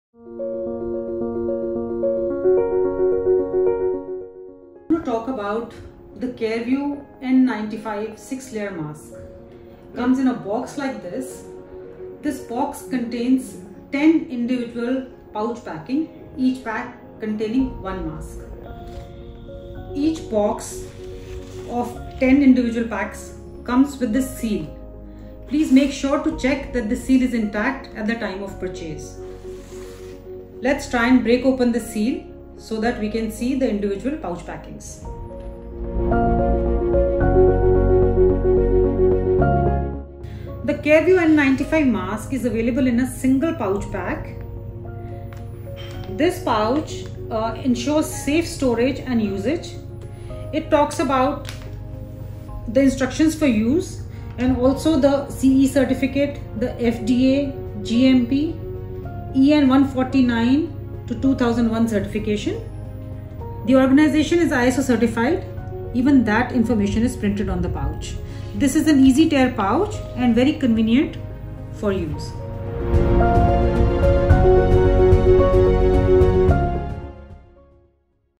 Care View N-95 Mask Packaging
Care View N-95 mask comes in a single piece pouch packaging which is safe for usage and storage purpose. After use, the mask can be stored in this pouch for repetitive usage. Also, a premium box packaging of 10 pouches is available as well for bulk selling.